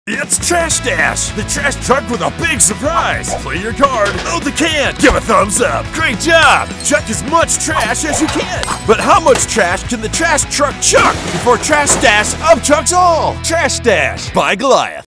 Trash Stash :15
It’s trash day! Place the pieces inside the garbage truck and give his arm a twist! Will you be safe, or will he chuck it up? It’s Trash Stash!

Available online and in stores.